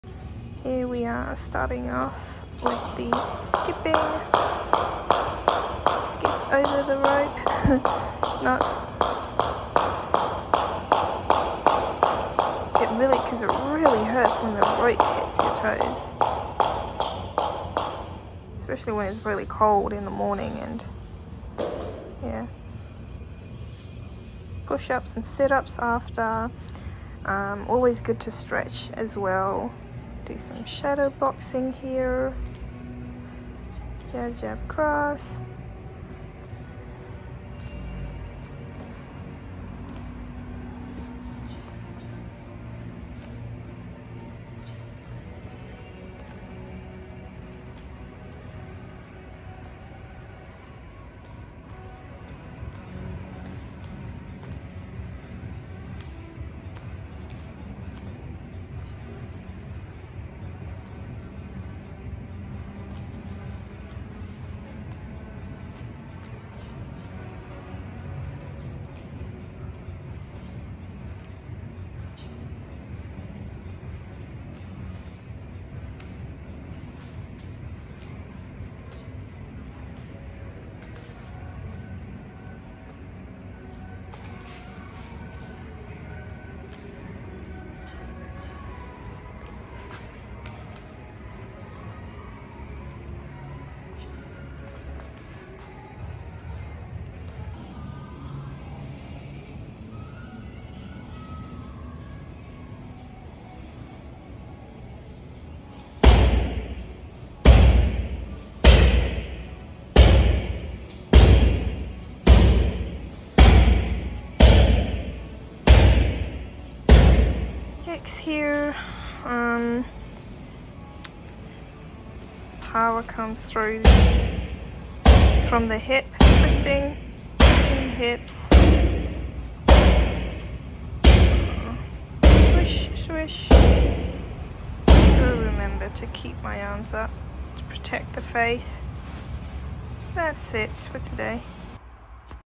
Mini muay thai vlog, tracting progress
Warming up with skipping, shadow boxing, practicing, kicking with bags